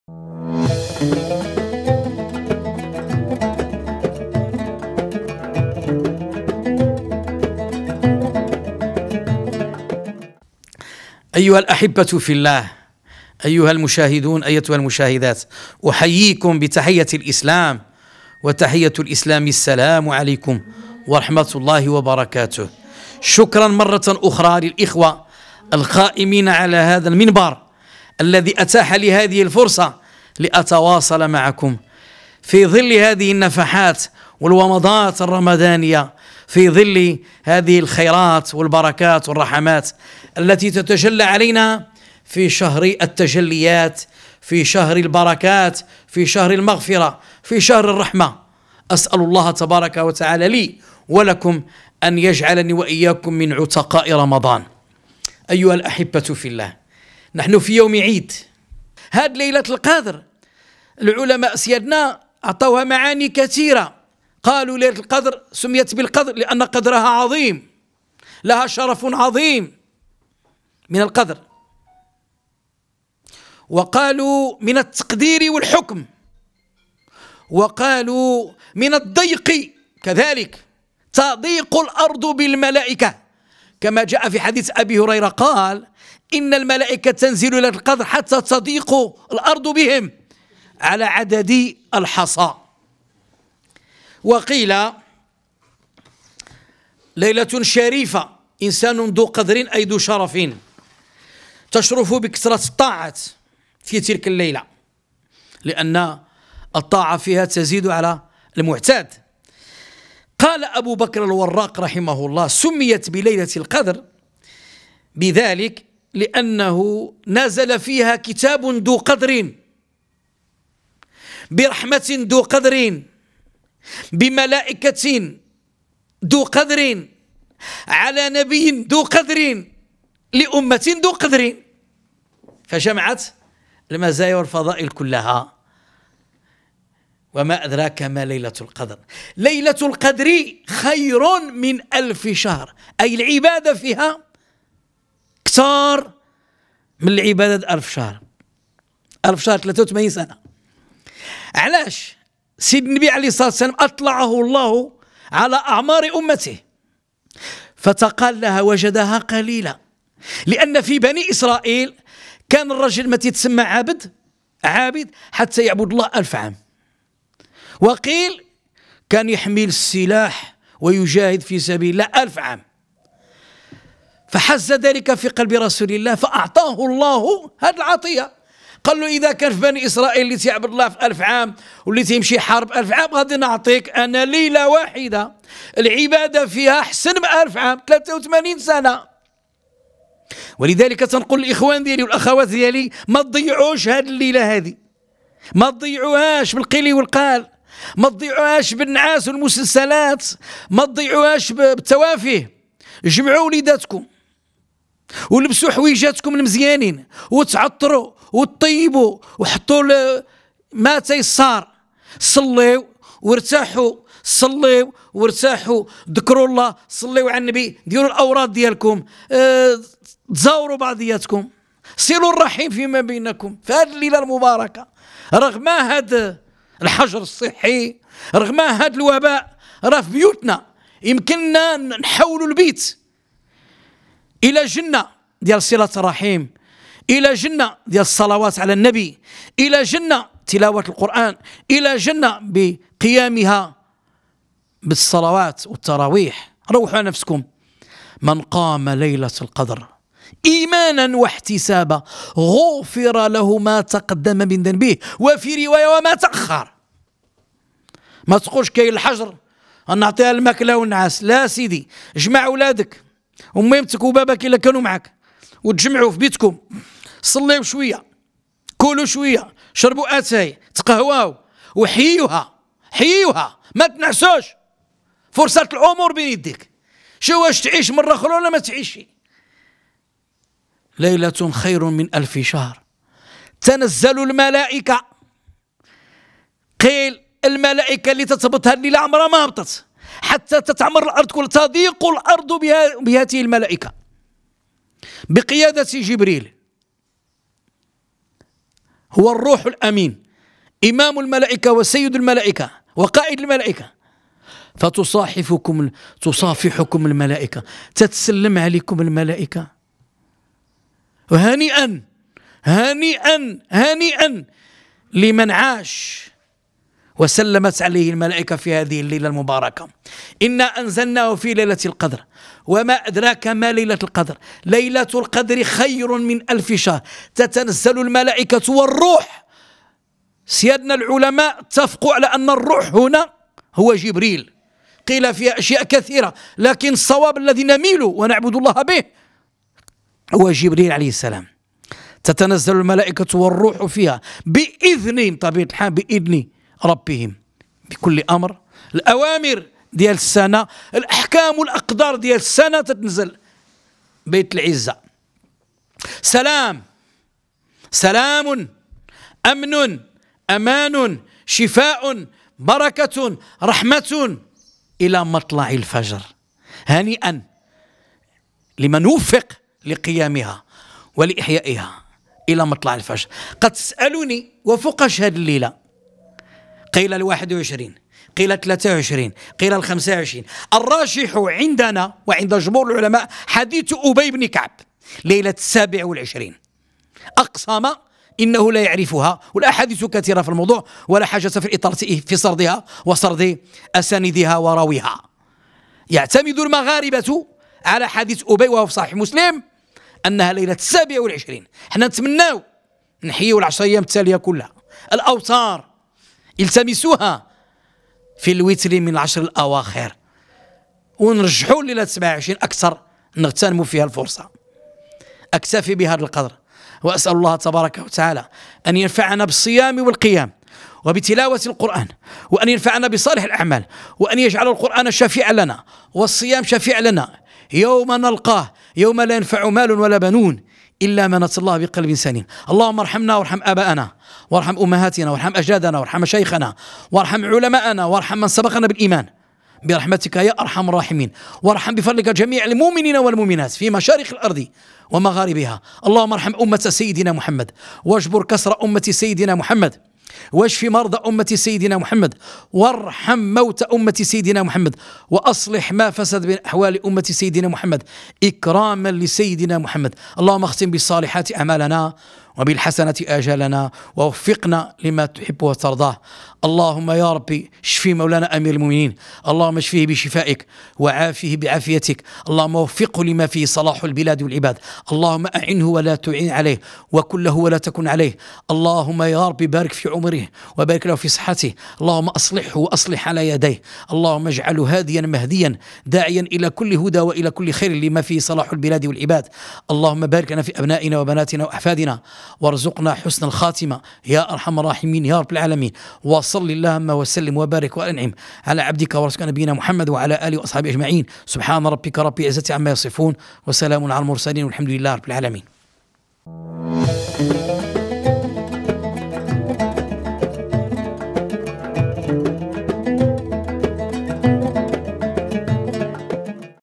العلامة الفاسي "محمد بنموسى" يكشف معاني ودلالات ليلة القدر:"هي خير من ألف شهر"
من خلال نفحات رمضانية ، قدم العلامة الفاسي "محمد بنموسى"، درسه حول ليلة القدر.